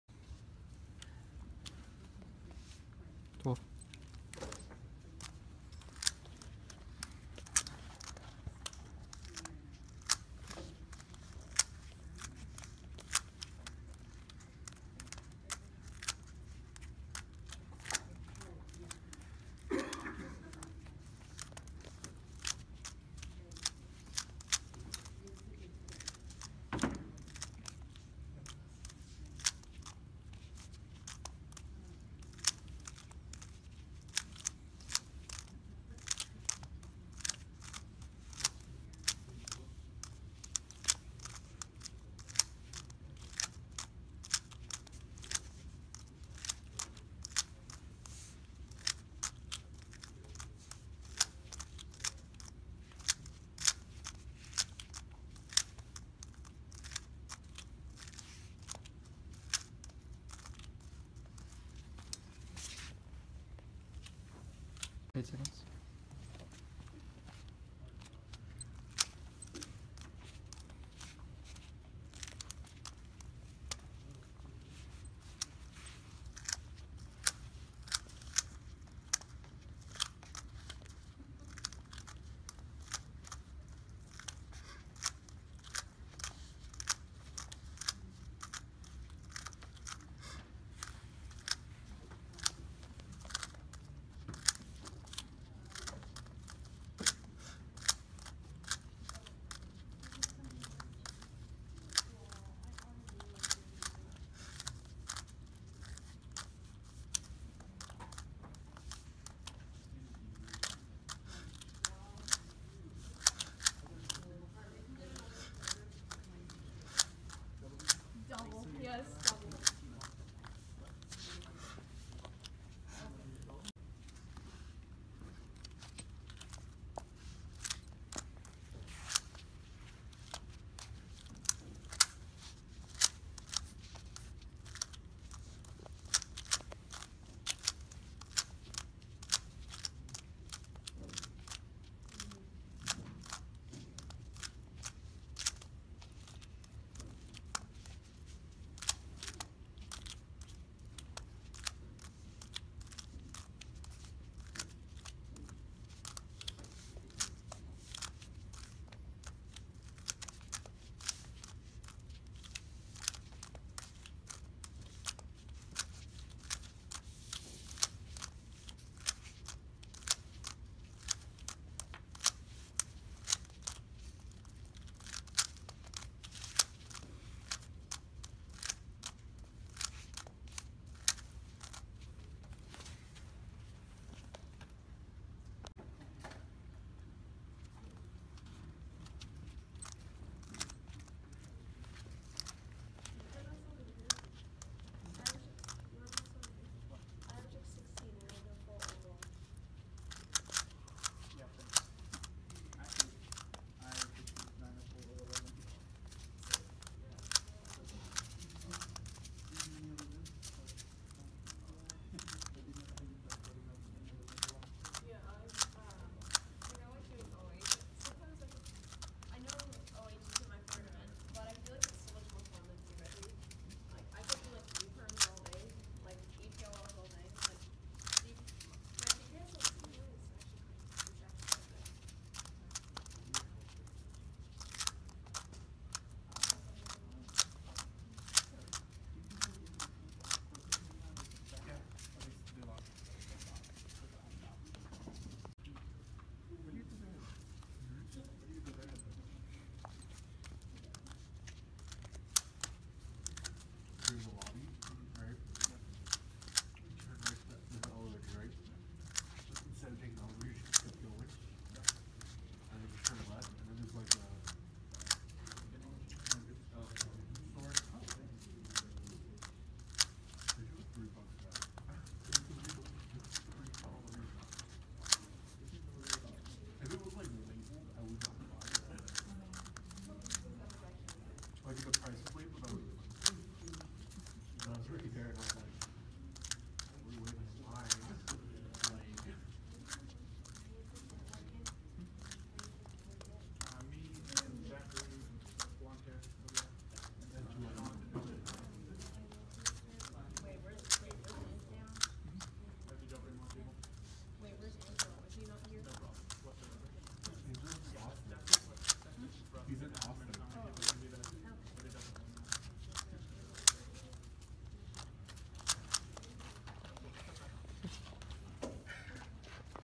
55.91 official feet average with ZZ
58.59, (52.43), 56.55, 52.60, (1:07.71) = 55.91 avg

Winning average at Southern Championships 2018

ZZ is lit

Thanks Zach White for filming and judging!